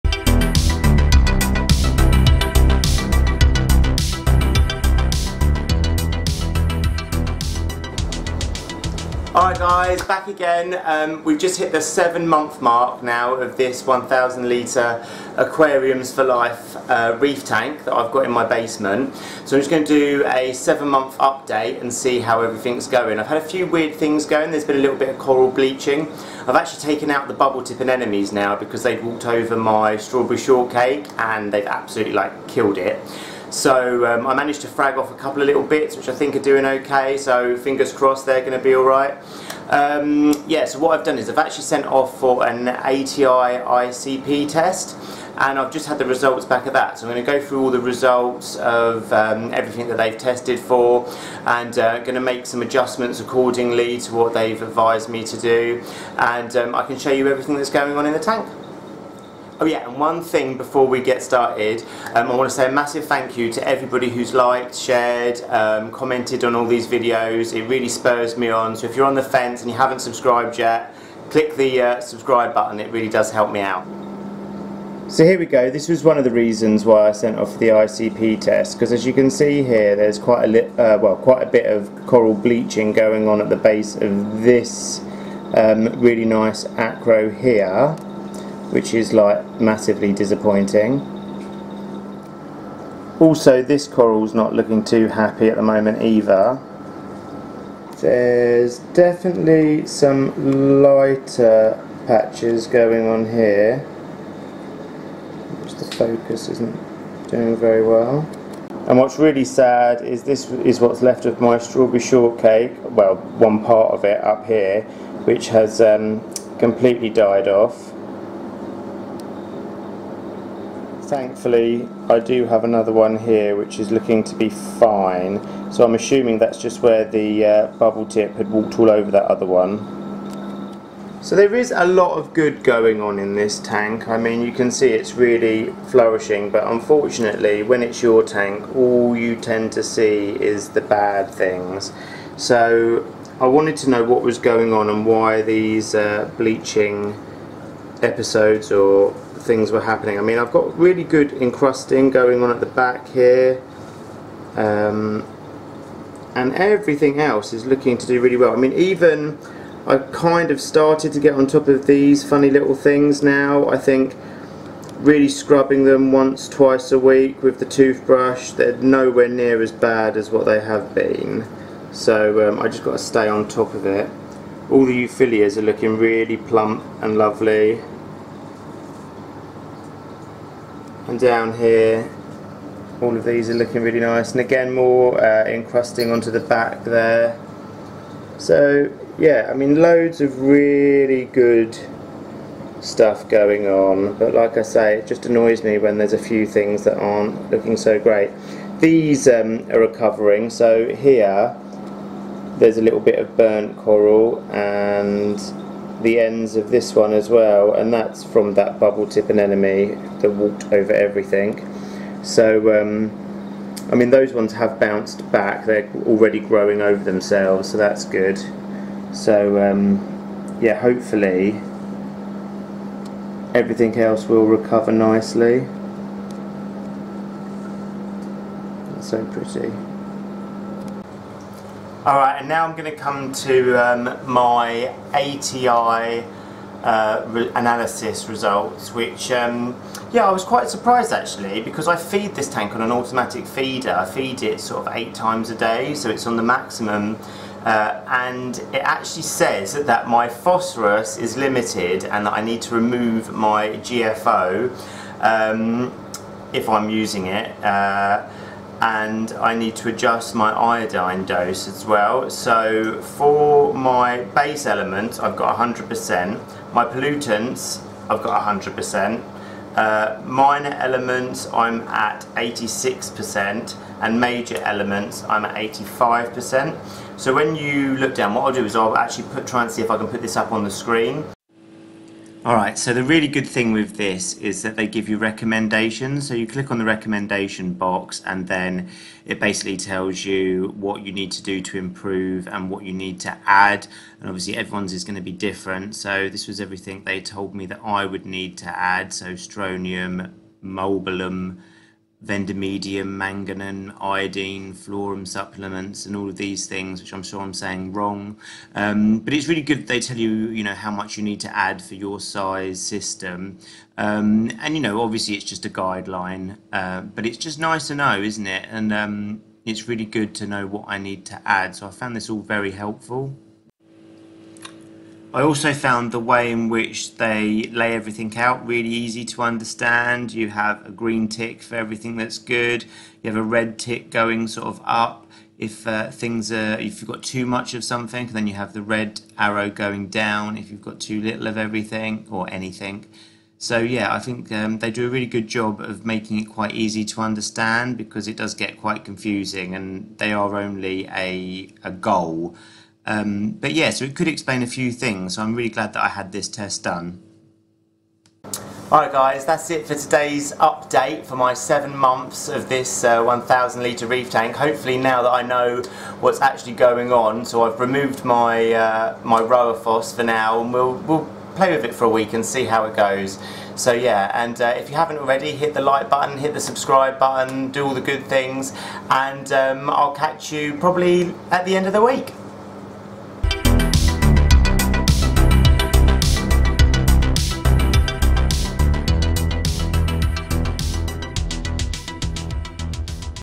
7 Month, update on my 1000 litre Reef Tank and the results of my ATI ICP test. Water test analysis.
In this update i have had a ATI ICP test performed on my tank to have a in-depth look at my water quality.

Here is a link to Get your own ATI test.